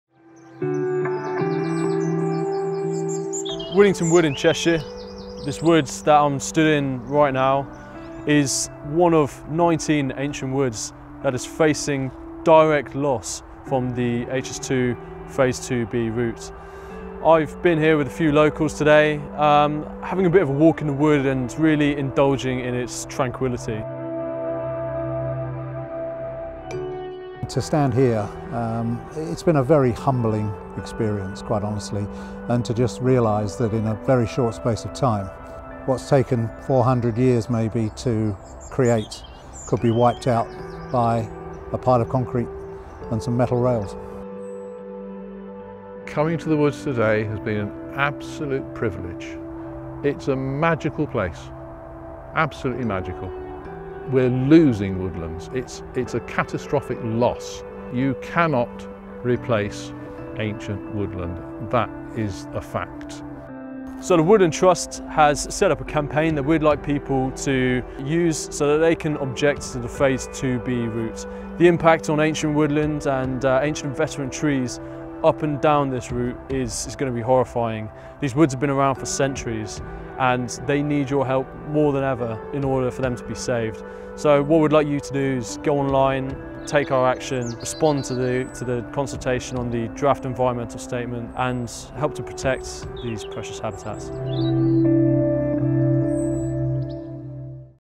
HS2 Ancient Woodland Threat – Winnington Wood
Right now, the biggest single threat from development to ancient woodland is the HS2 rail project. Up and down the country ancient woods and trees face the axe to make way for the high speed train line. Currently 108 ancient woods are threatened with loss or damage from both Phase 1 and Phase 2 of the project.